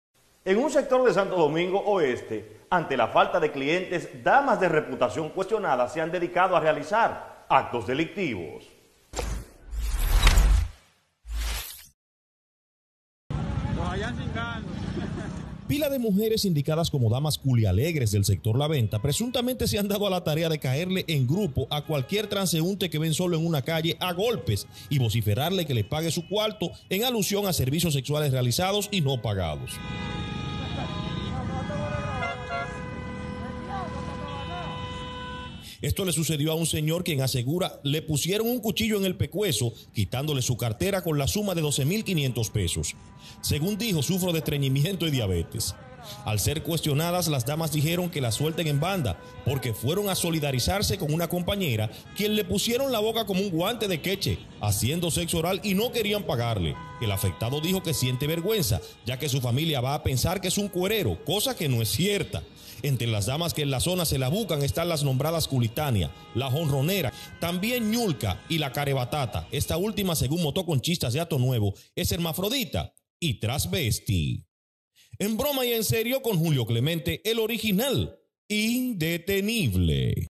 En Villa Aura: Sexoservidoras "Dan Golpia" a Hombre de 67 años
Un grupo de mujeres sindicadas como "Damas culialegres" del sector La venta presuntamente se han dado a la tarea de "Caerle a Golpes en grupo" a cualquier transeúnte que ven solo en una calle vociferandole "Que le pague su cualto" en alusión a servicios sexuales realizados y no pagados.